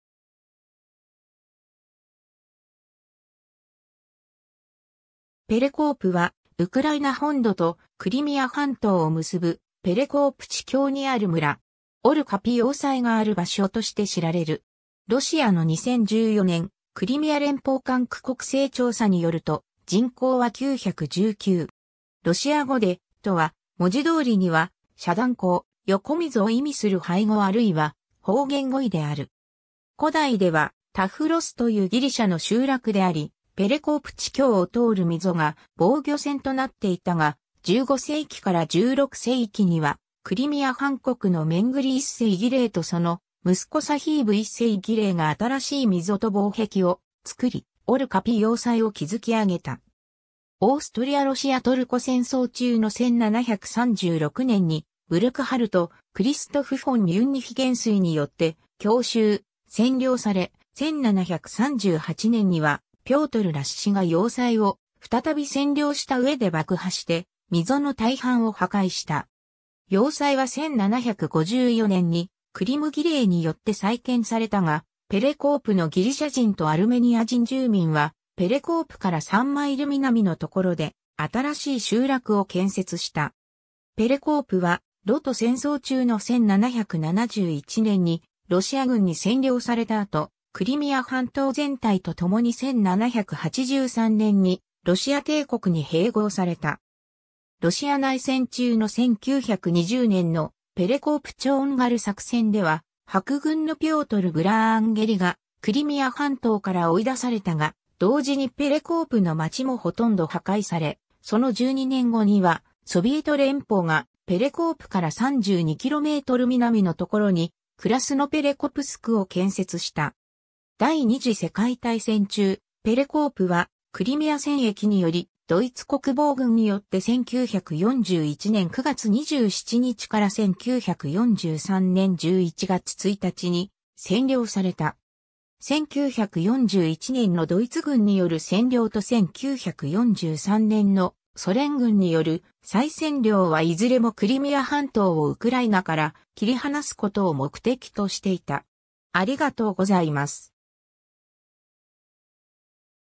ペレコープ
ペレコープ（ウクライナ語: Перекоп、ロシア語: Перекоп、クリミア・タタール語: Ор Къапы、ギリシア語: Τάφρος）は、ウクライナ本土とクリミア半島を結ぶペレコープ地峡にある村。
オル・カピ要塞がある場所として知られる。
ロシアの2014年クリミア連邦管区国勢調査によると、人口は919。
ロシア語で перекоп とは文字通りには〈遮断溝、横溝〉を意味する廃語あるいは方言語彙である。
 古代ではタフロス（Τάφρος）というギリシャの集落であり、ペレコープ地峡を通る溝が防御線となっていたが、15世紀から16世紀にはクリミア・ハン国のメングリ1世ギレイとその息子サヒーブ1世ギレイが新しい溝と防壁を作り、オル・カピ要塞を築き上げた。
オーストリア・ロシア・トルコ戦争中の1736年にブルクハルト・クリストフ・フォン・ミュンニヒ元帥によって強襲、占領され、1738年にはピョートル・ラッシが要塞を再び占領した上で爆破して、溝の大半を破壊した。
要塞は1754年にクリム・ギレイによって再建されたが、ペレコープのギリシャ人とアルメニア人住民はペレコープから3マイル南のところで新しい集落（現アルミャーンシクにあたる）を建設した。
ペレコープは露土戦争中の1771年にロシア軍に占領された後、クリミア半島全体とともに1783年にロシア帝国に併合された。
 ロシア内戦中の1920年のペレコープ＝チョーンガル作戦では白軍のピョートル・ヴラーンゲリがクリミア半島から追い出されたが、同時にペレコープの町もほとんど破壊され、その12年後にはソビエト連邦がペレコープから32km南のところにクラスノペレコプスクを建設した。
 第二次世界大戦中、ペレコープはクリミア戦役によりドイツ国防軍によって1941年9月27日から1943年11月1日に占領された。
1941年のドイツ軍による占領と1943年のソ連軍による再占領はいずれもクリミア半島をウクライナから切り離すことを目的としていた。